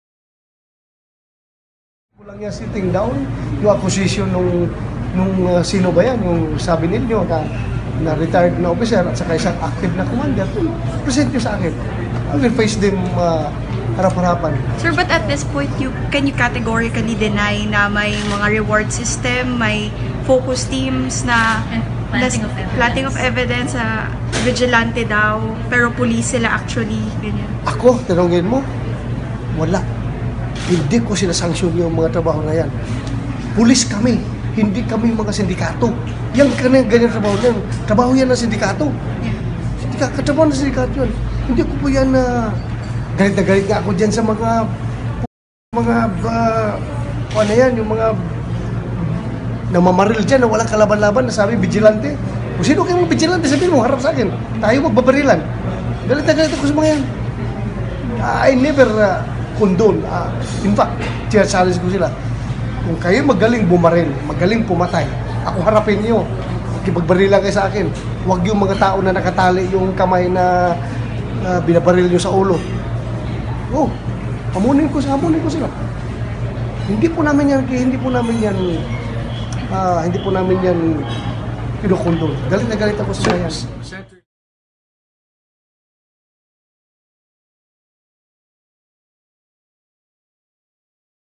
Dela Rosa says cops are not vigilantes, denies Reuters report
Amid persistent claims that the police have become President Duterte’s “killing machine” for his brutal drug war, the Philippines’ top cop Director General Ronald “Bato” Dela Rosa strongly denied that he sanctions killings as part of the government’s anti-illegal drug campaign. Video by Julianne Love de Jesus